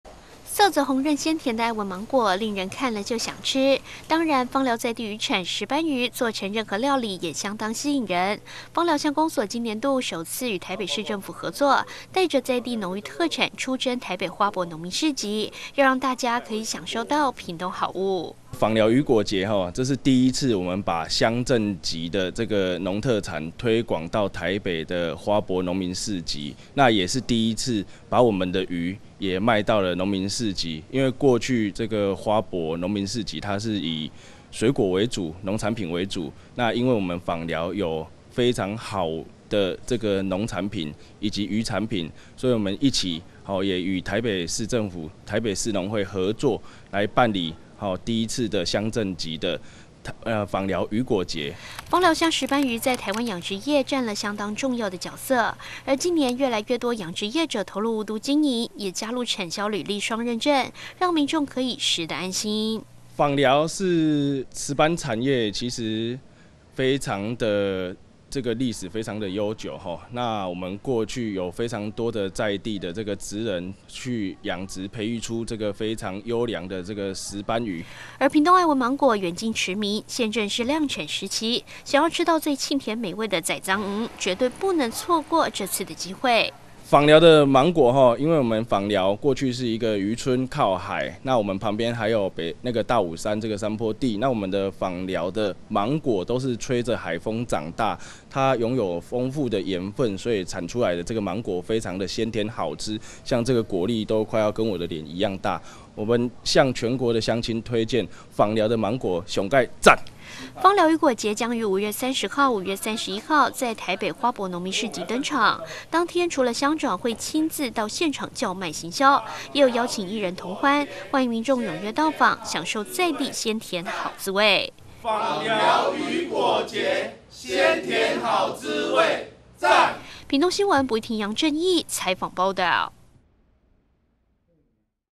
109 0529 枋寮「漁菓」出征台北 推廣在地鮮甜好滋味
枋寮鄉公所第一次與台北市政府合作，將枋寮鄉的農漁產，石斑魚及愛文芒果，出征至台北花博農民市集，舉辦枋寮漁菓節銷售大會，希望推廣在地鮮甜好滋味的同時，也能藉機會行銷「屏東」。